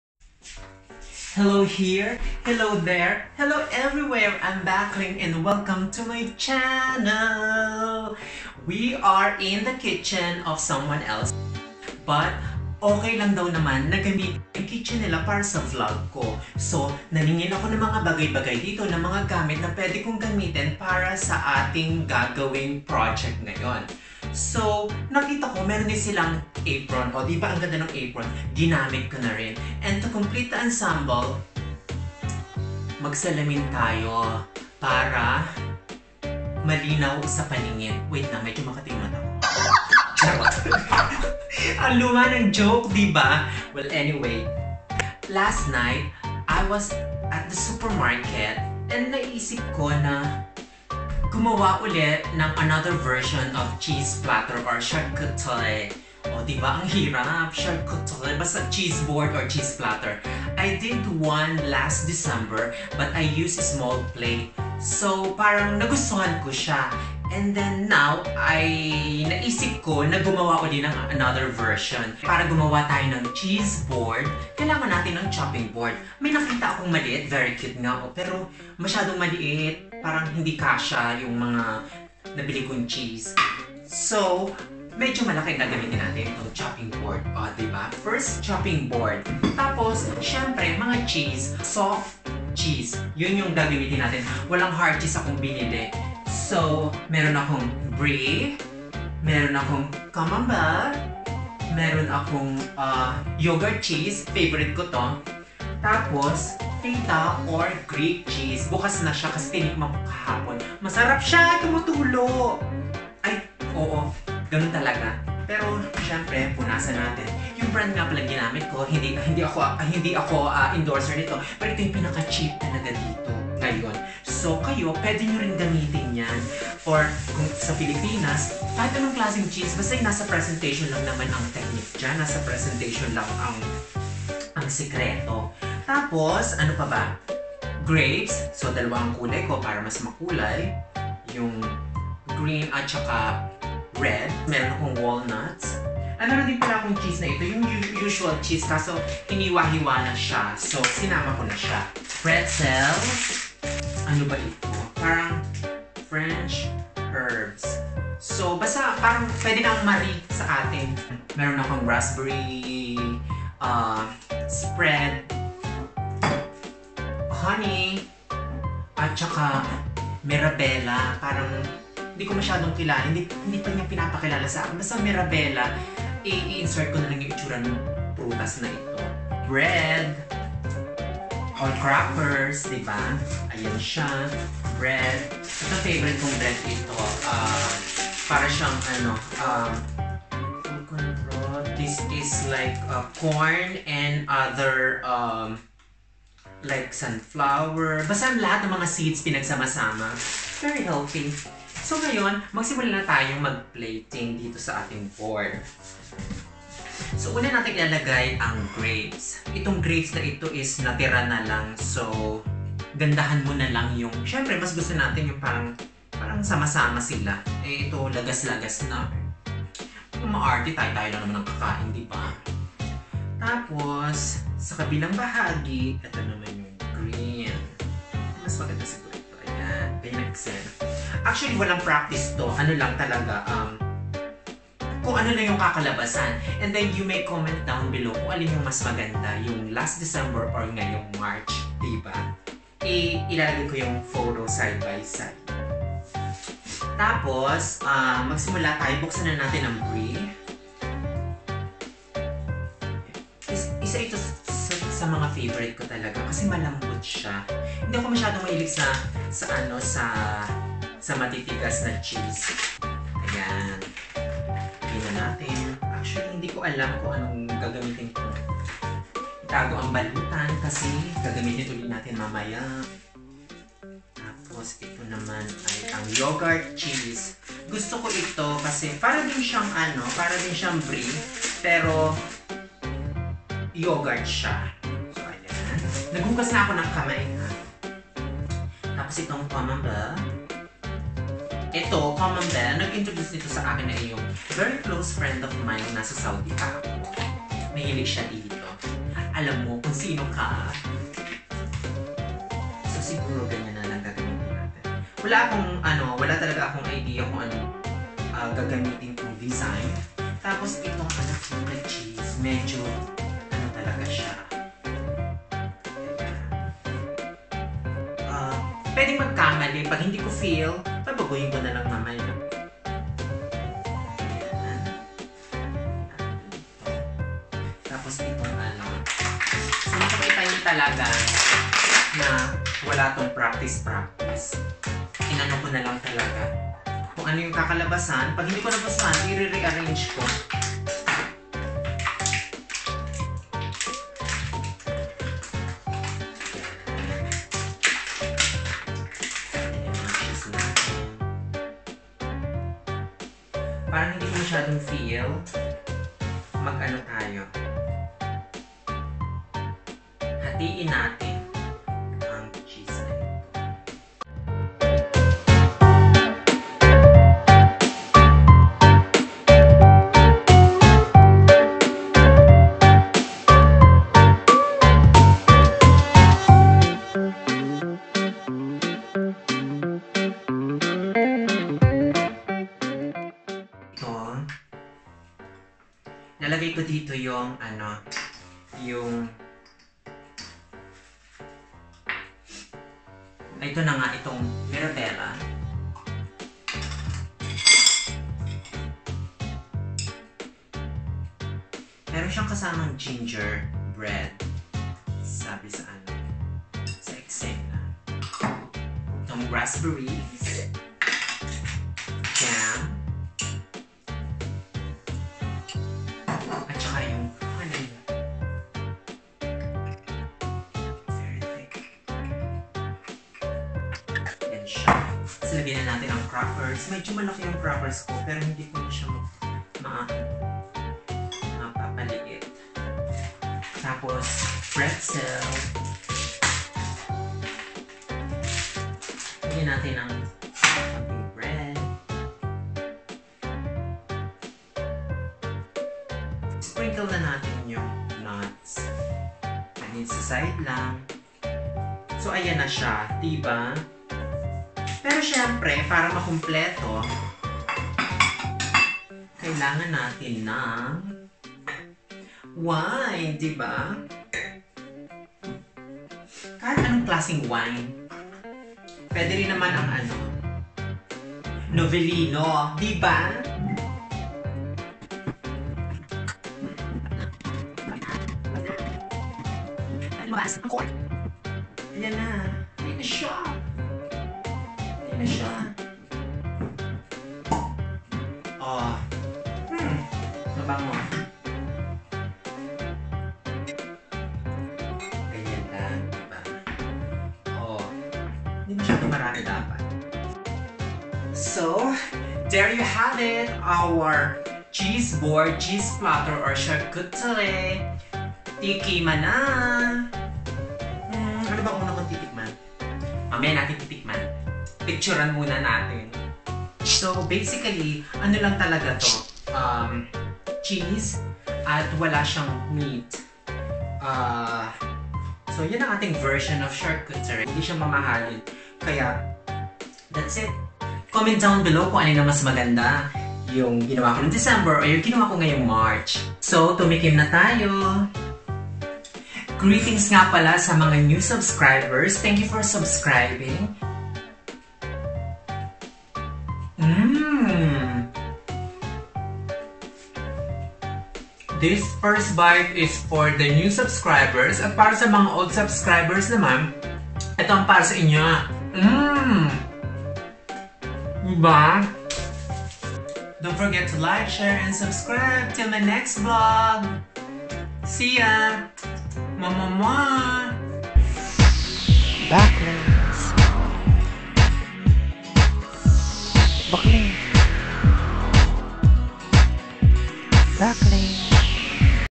How to make a simple and affordable Charcuterie or Cheese Platter or Cheeseboard
My simple, affordable and very easy to make Charcuterie or Cheese Board.

Last December 2020, I made a cheese platter and I liked it. Now this is another version and I am happy to share it with you.